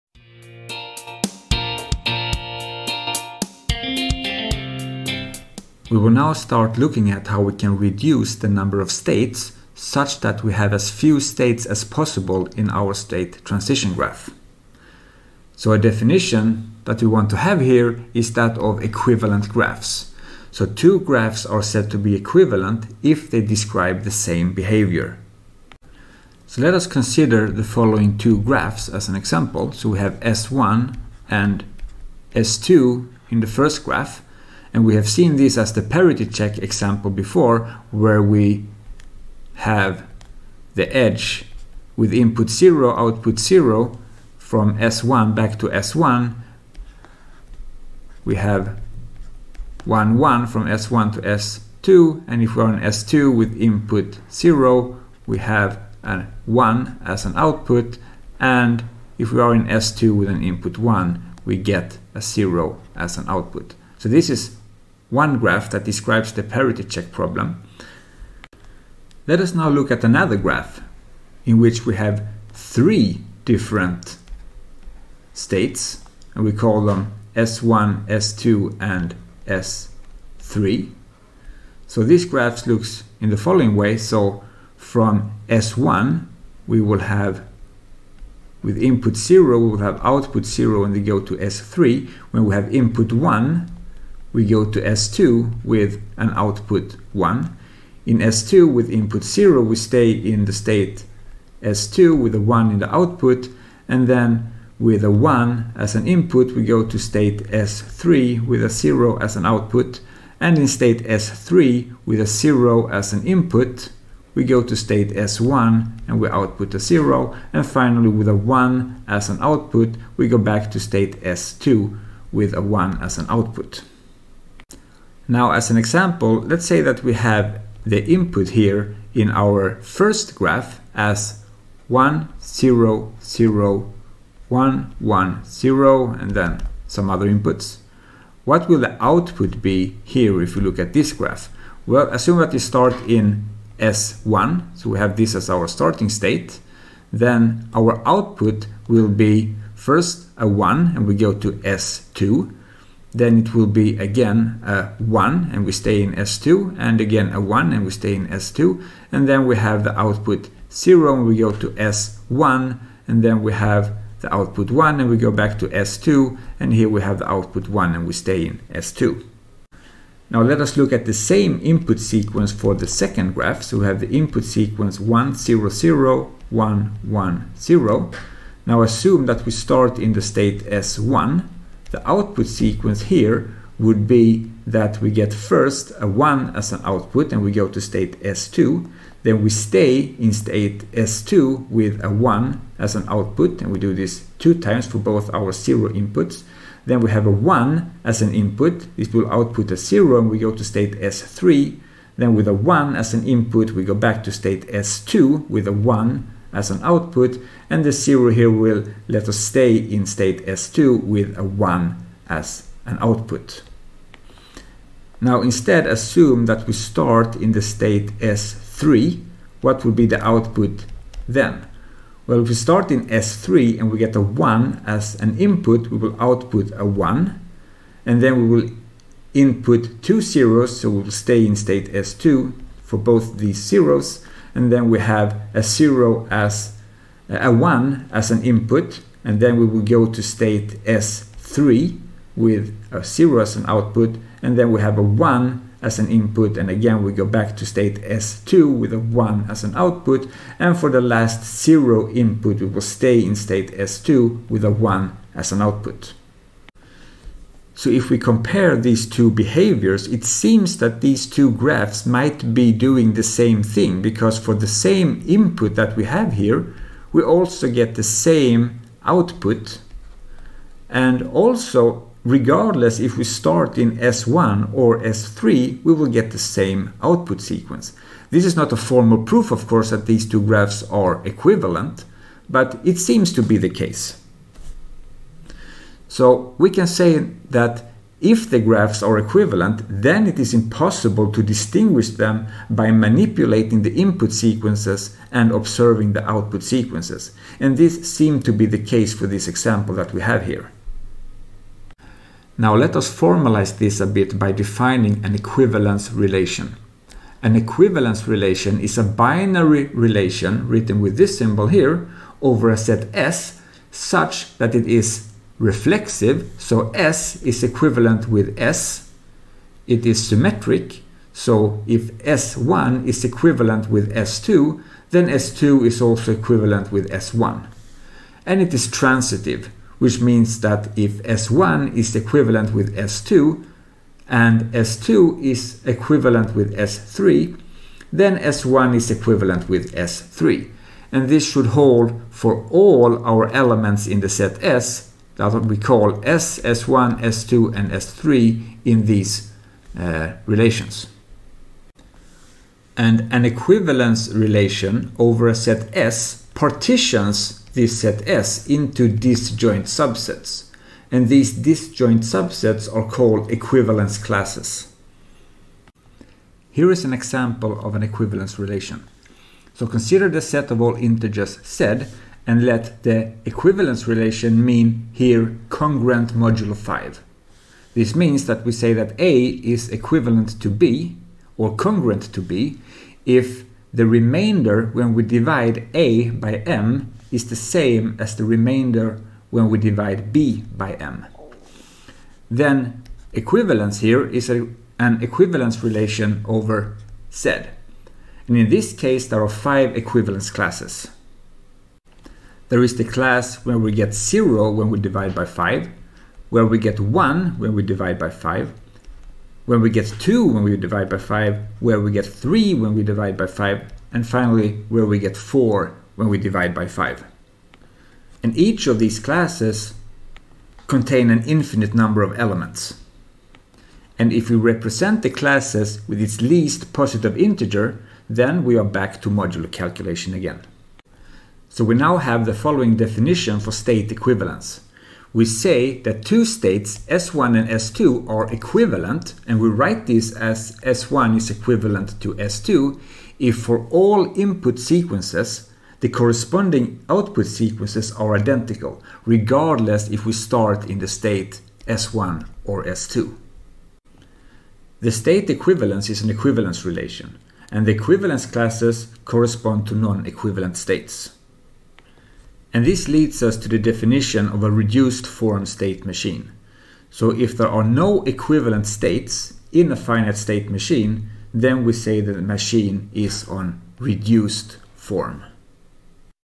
9.1 Equivalent Graphs and Equivalent States
We define equivalent states, define and give examples of equivalence relations, and define what we mean by equivalent states. If we do not have any equivalent states, then the graph is on reduced form.